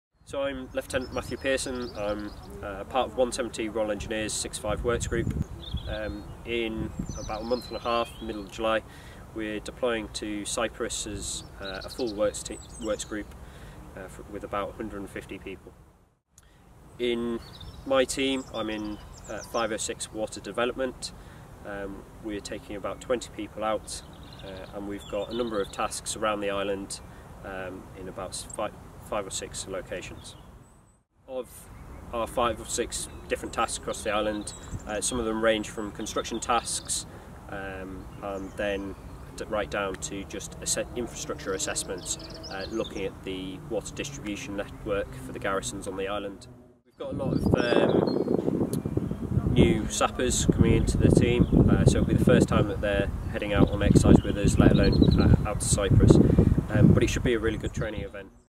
Lieutenant Matthew Pearson from 170 Infrastructure Support Engineer Group
Reservist Matthew Pearson from 170 Engineer Group speaks about completing a training exercise in Cyprus this summer